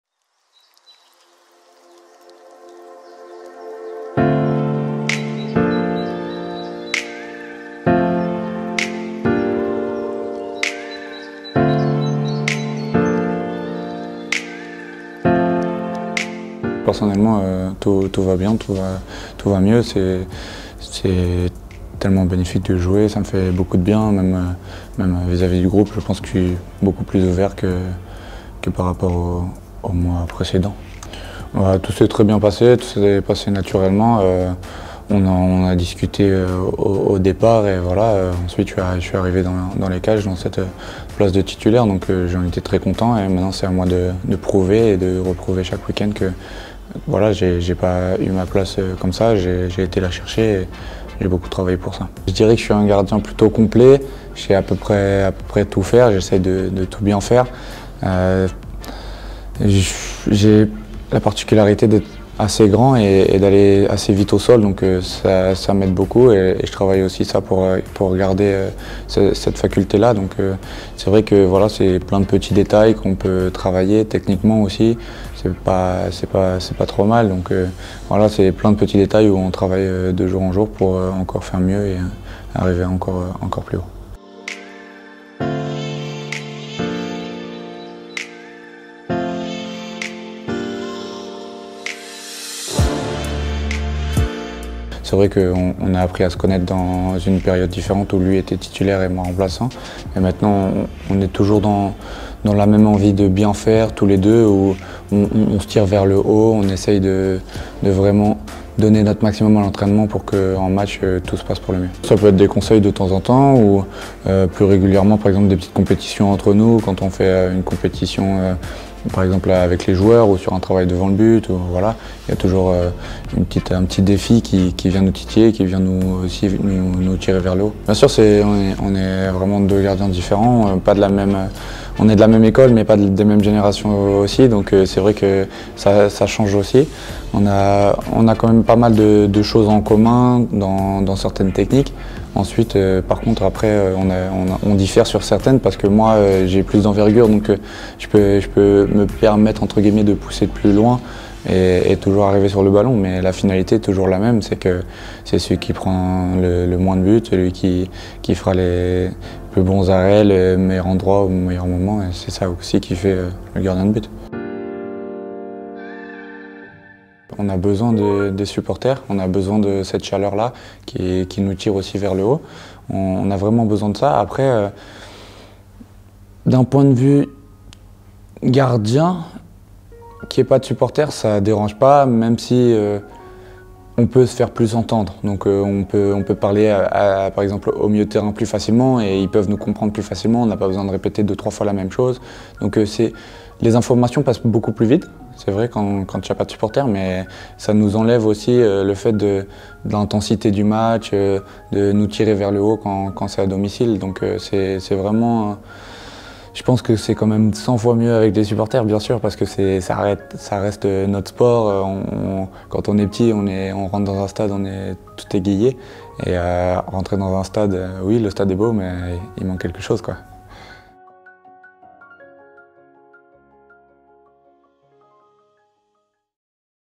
Le portrait de la semaine - E16 - Remy Descamps
🎥 De nouveau titulaire dans les cages zébrées, Remy Descamps a appris à être patient. Désormais sur le terrain chaque semaine, il ne lui manque qu'une seule chose : la présence des supporters🔥